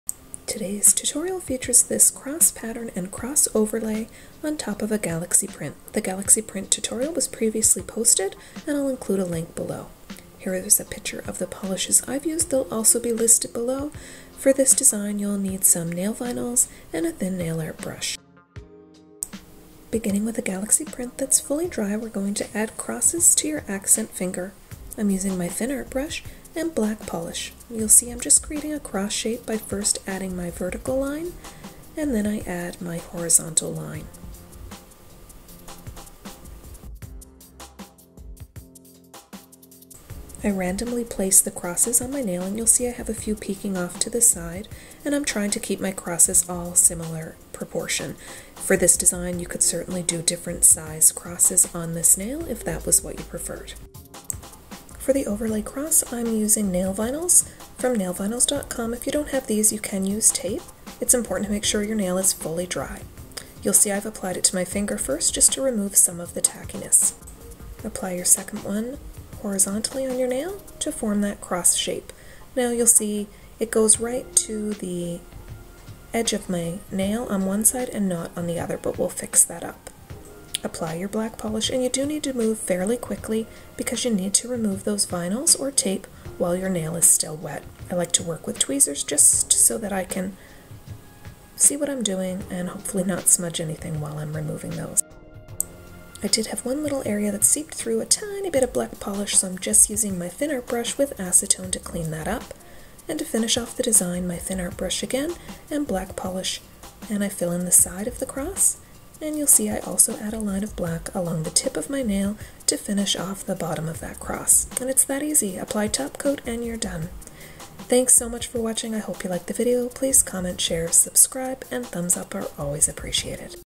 ChristabellNails Galaxy and Crosses Nail Art Tutorial
Supplies needed are polish (listed below) (plus base and top coat), thin art brush (or toothpick), nail vinyls, makeup sponge, paper towel, and acetone.

Polishes Used:  Sally Hansen Hard as Nails Black Heart; Maybelline Color Show Styled Out (base blue), Denim Dash (blue), and Porcelain Party (white); Colour Gossip Paparazzi Ready (pink); Sinful Colors Dream On (fuchsia); and China Glaze Fairy Dust (topcoat).

Nail Vinyls or @teismom on Instagram